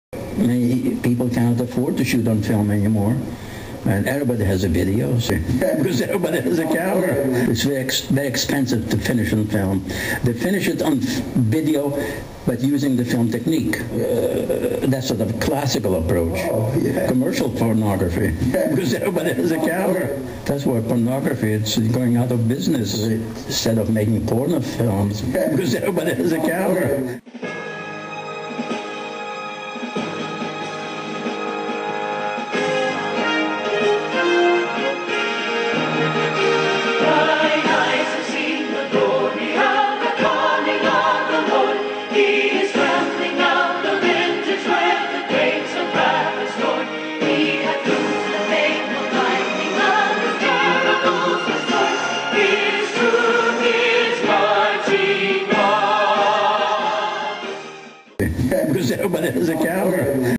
Everybody Has A Camera
Visualization in the 21st C: The Barbie Video camera...will it replace the handheld Bolex?